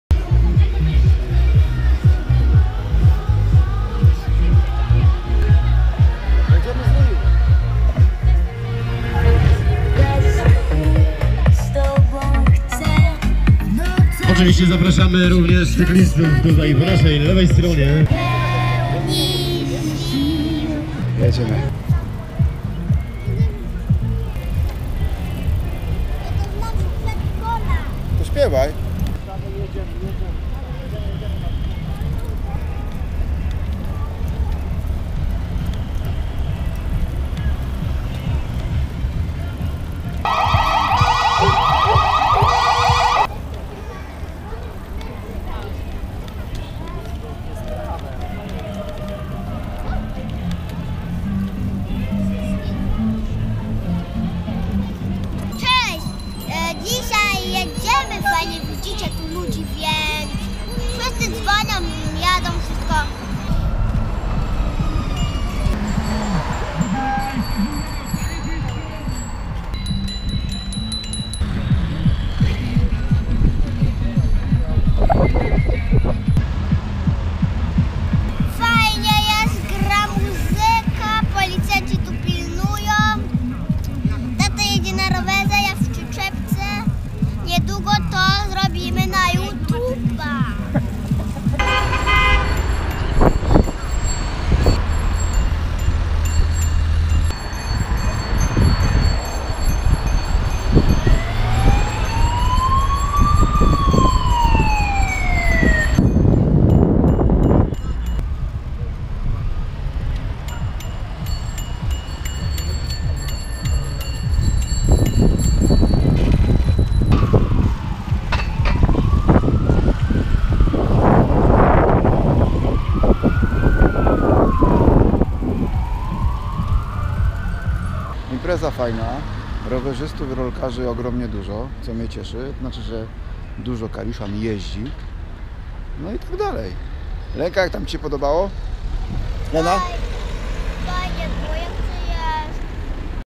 V Rajd rolkowo-rowerowy "Kalisz świeci przykładem"
Lenka podczas rajdu. Świetna atmosfera i dużo zabawy!